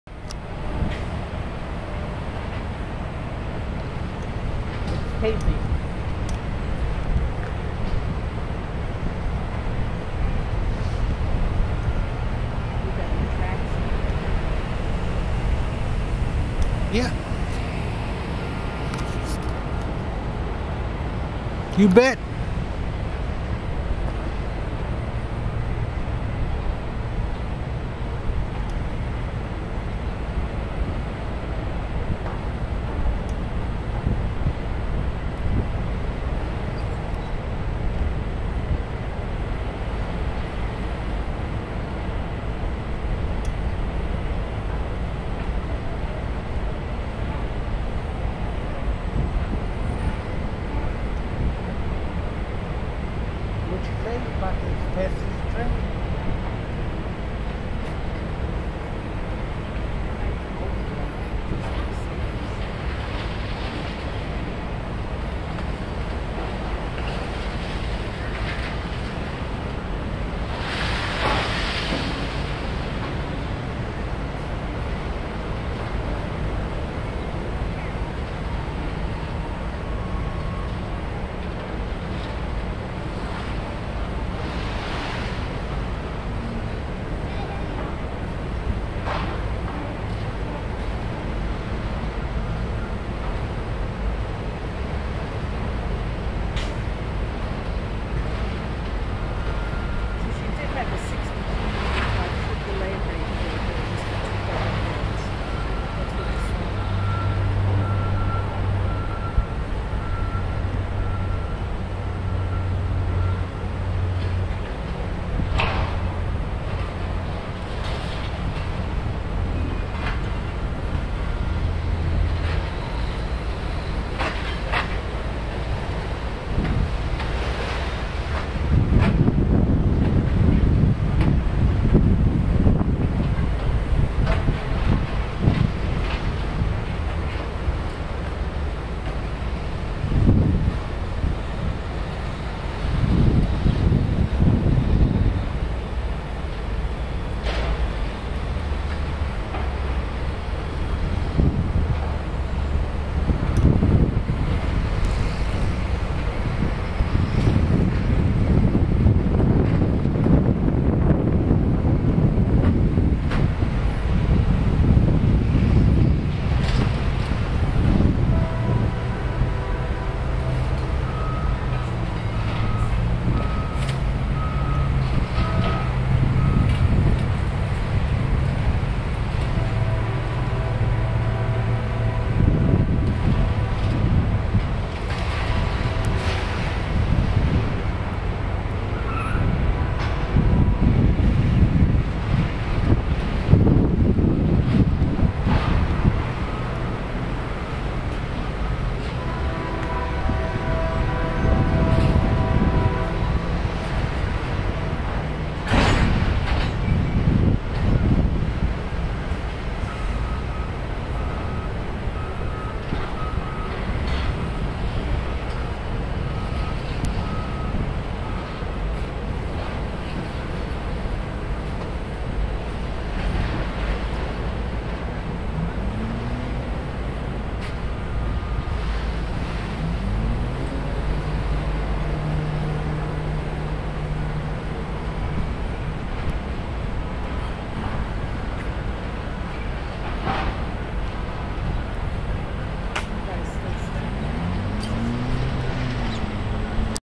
BNSF Swing Bridge Over the Columbia River @ Vancouver WA Closing
BNSF swing bridge over the Columbia River @ Vancouver WA closing after allowing sailing boat to pass thru it